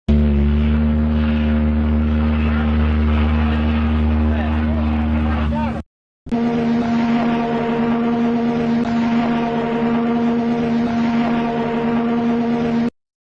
Whistle sound of the SS Berlin and the RMS Majestic
I will show you guys the whistle sound of the SS Berlin and the RMS Majestic.